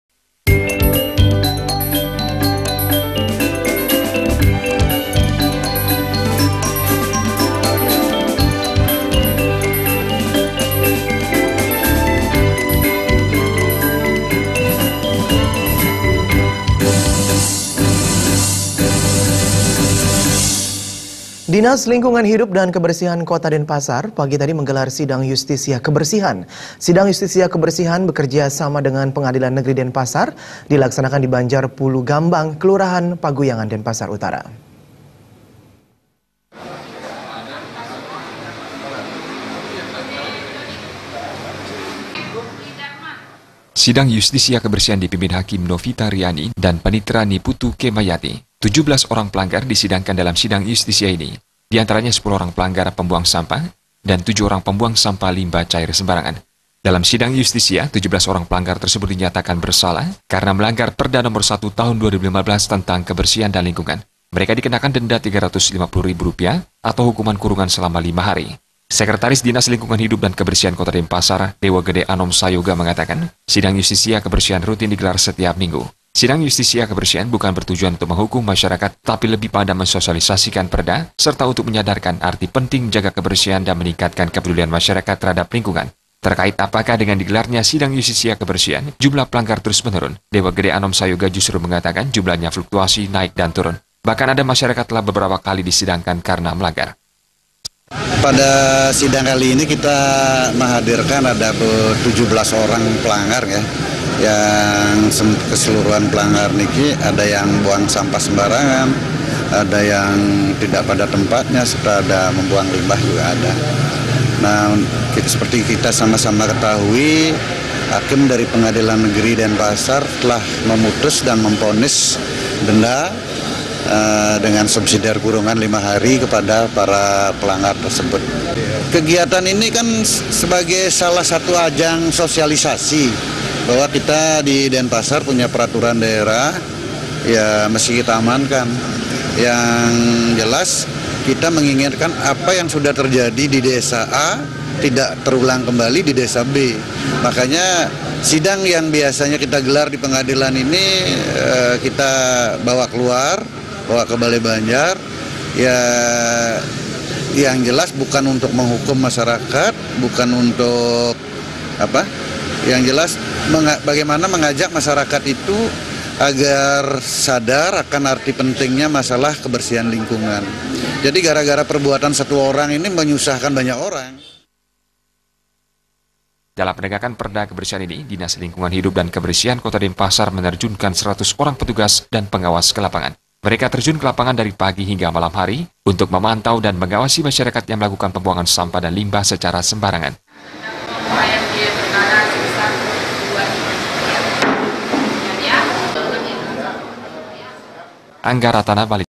17 PELANGGAR JALANI SIDANG YUSTISIA
DINAS LINGKUNGAN HIDUP DAN KEBERSIHAN KOTA DENPASAR,MENGGELAR SIDANG YUSTISIA KEBERSIHAN. SIDANG YUSTISIA KEBERSIHAN , BEKERJASAMA DENGAN PENGADILAN NEGERI DENPASAR, DILAKSANAKAN DI BANJAR PULUGAMBANG, KELURAHAN PEGUYANGAN, DENPASAR UTARA.